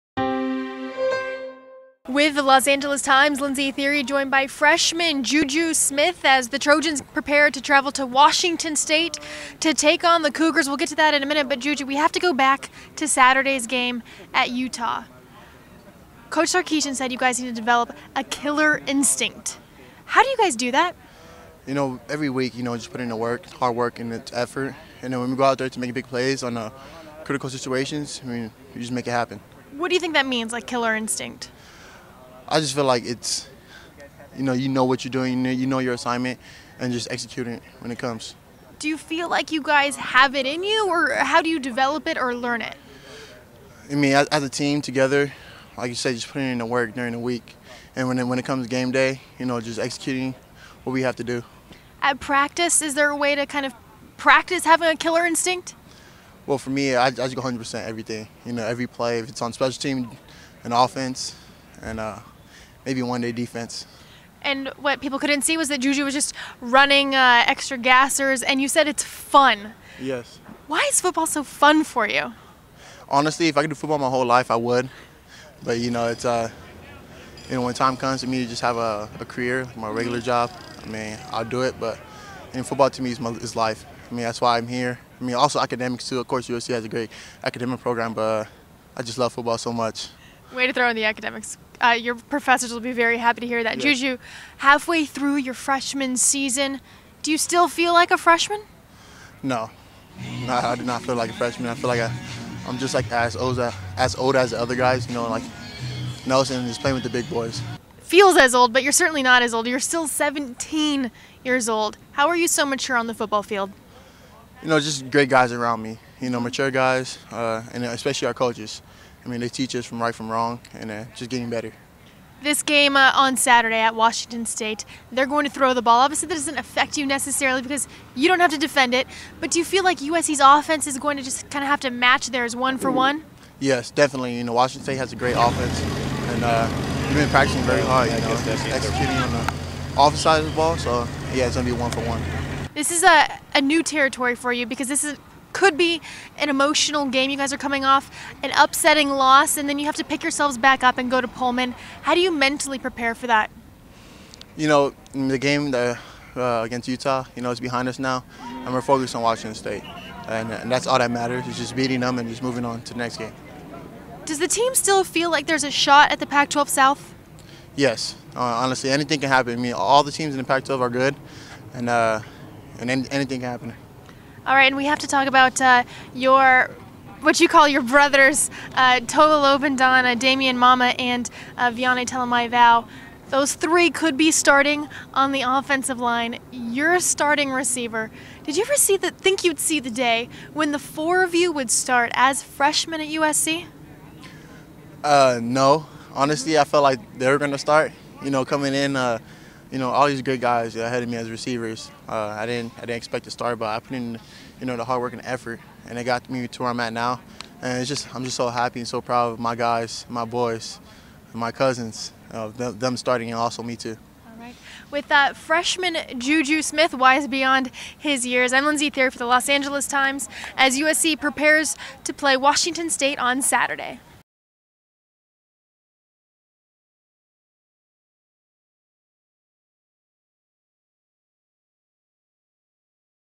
USC's JuJu Smith talks Washington State: USC Now
USC's JuJu Smith discusses Saturday's game at Washington State.